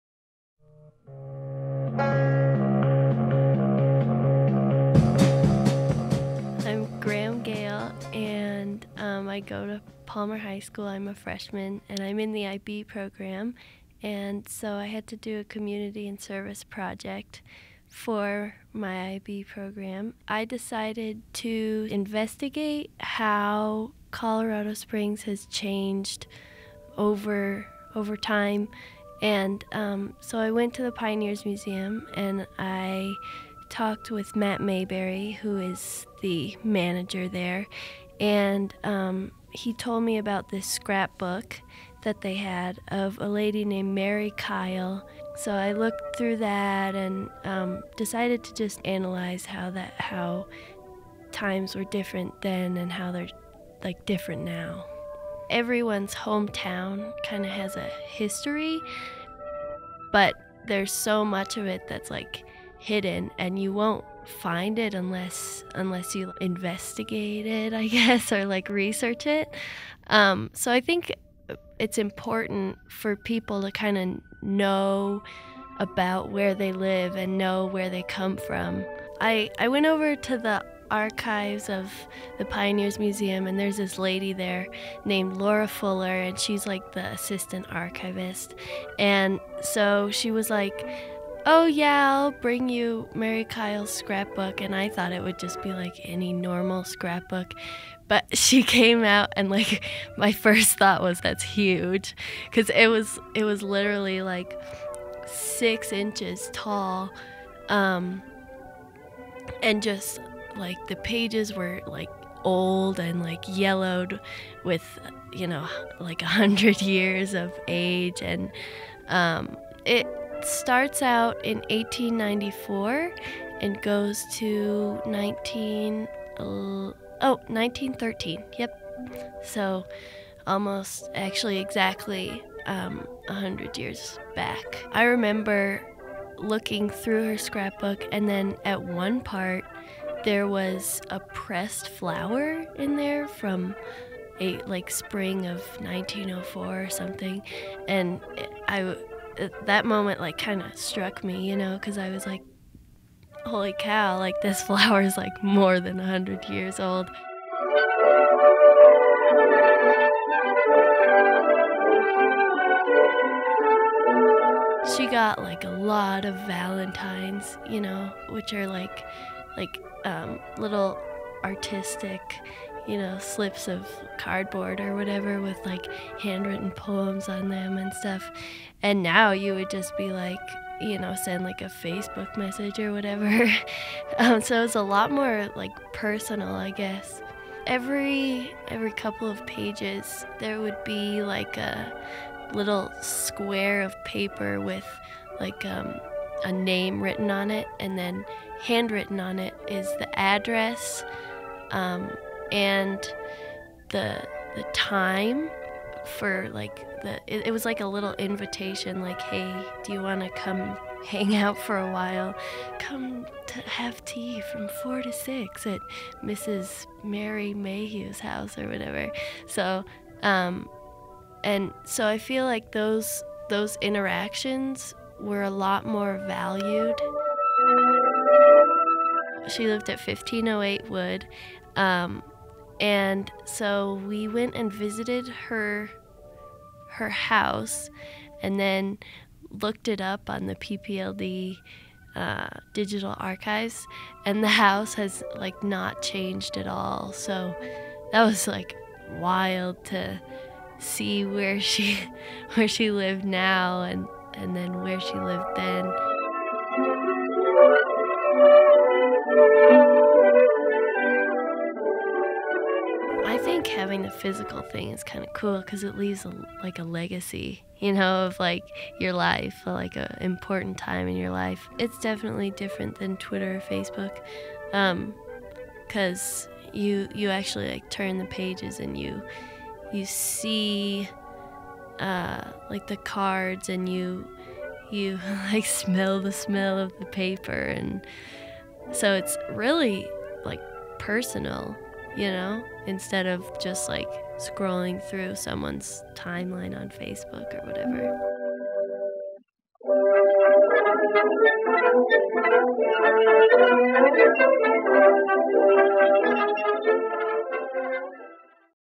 Mary Kyle's Century-Old Scrapbook
Palmer High School Student, Graham Gale, came to us with an idea to combine two of her interests - local history & public media - for a school project.  Graham visited the Colorado Springs Pioneers Museum and found the scrapbook of a young woman who lived in the rather different Colorado Springs of 100 years ago.